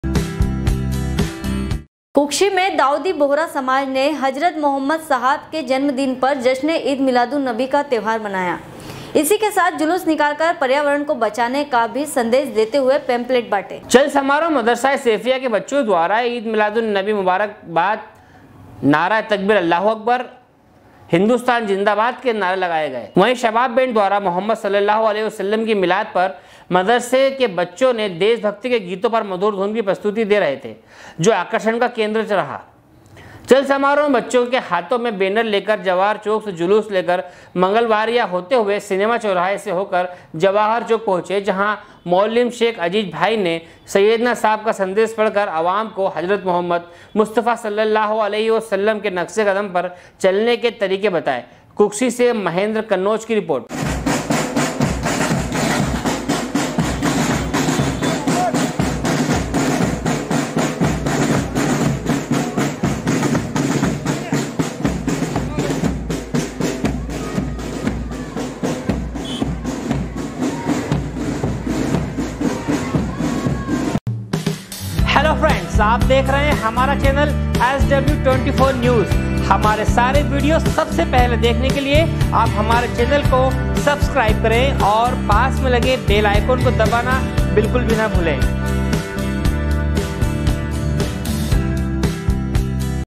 sw24news kukshi, 11.11.2019
कुक्षी में दाऊदी बोहरा समाज ने हजरत मोहम्मद साहब जन्मदीन पर जश्ने ईद मिलादुन्नबी का त्योहार मनाया कुक्षी से महेन्द्र कन्नौज की रिर्पोट#sw24news